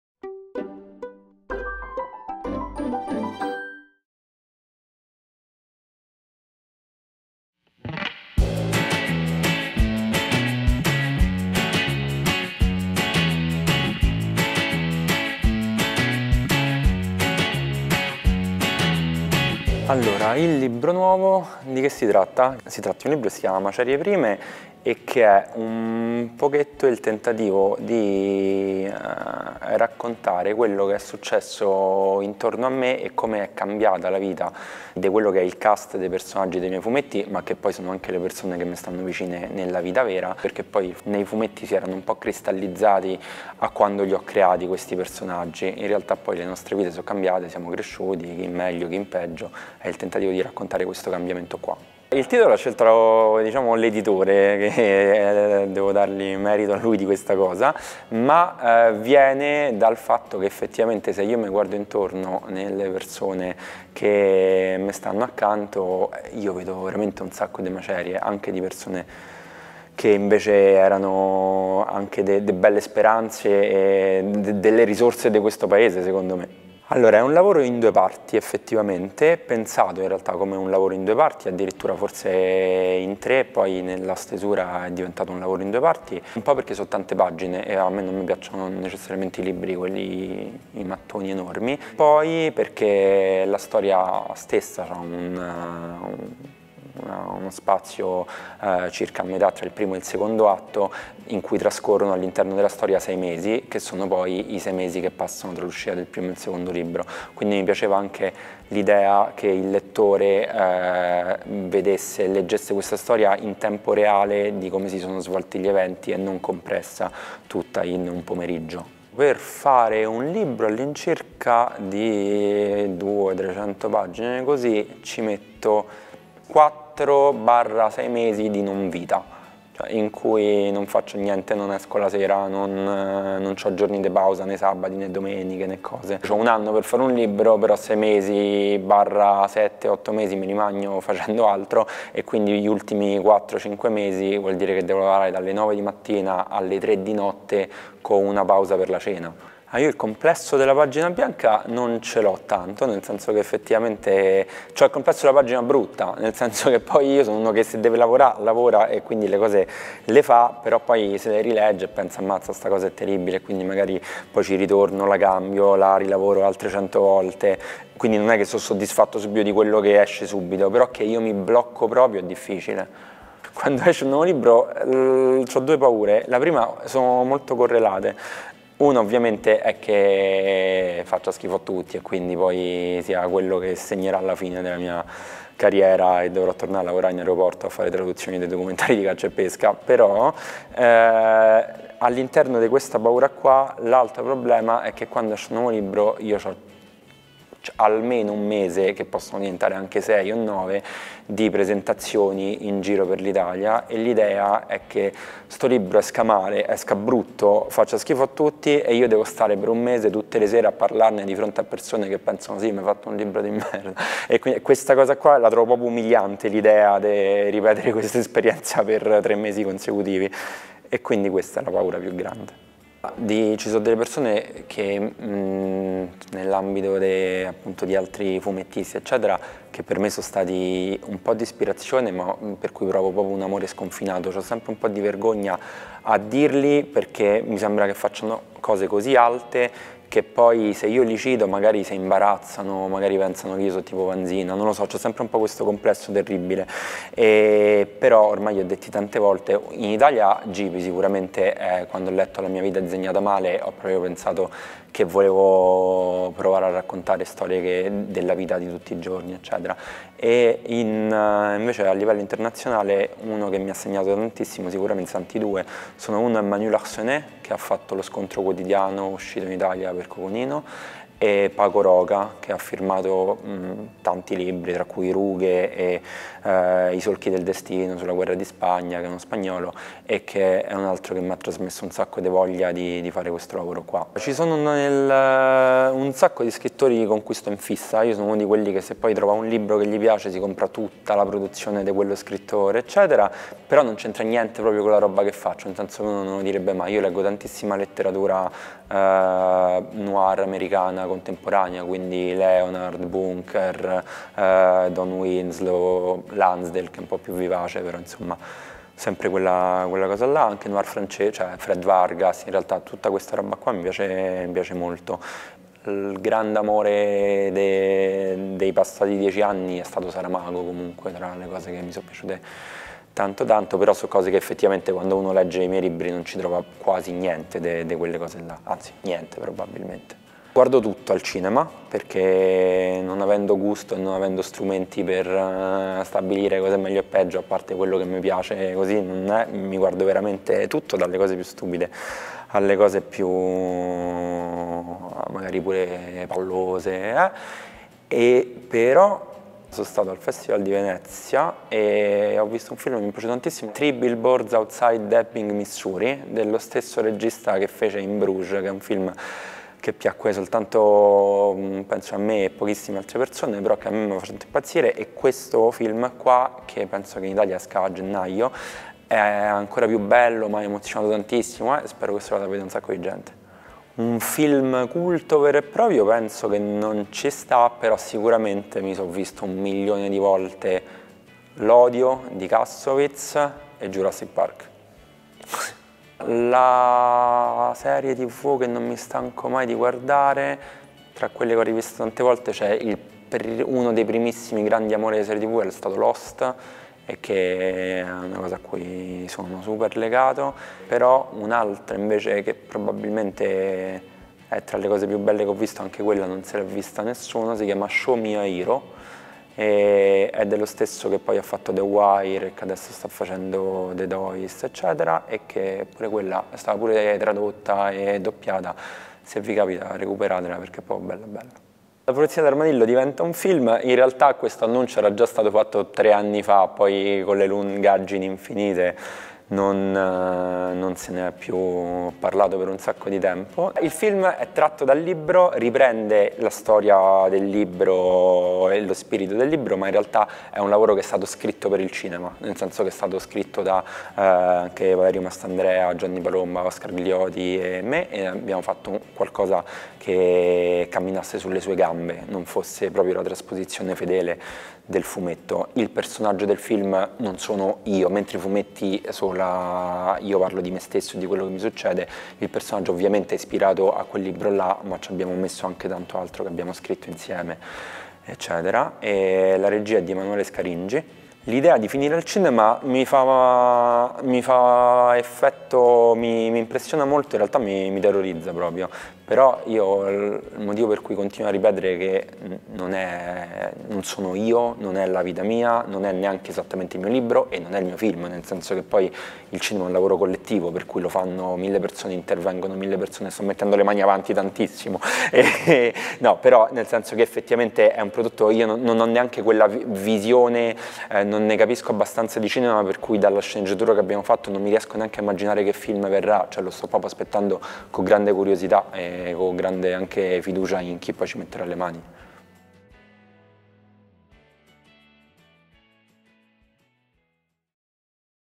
Zerocalcare - Macerie prime
“Macerie prime racconta com’è cambiata la vita dei personaggi dei miei fumetti, che poi sono anche le persone che mi stanno vicino nella vita reale”, spiega Zerocalcare intervistato al festival di Internazionale a Ferrara. “Le nostre vite sono cambiate, in meglio e in peggio. Quando mi guardo intorno vedo un sacco di macerie”. L’autore di fumetti romano parla del suo libro e racconta le sue paure e le sue passioni.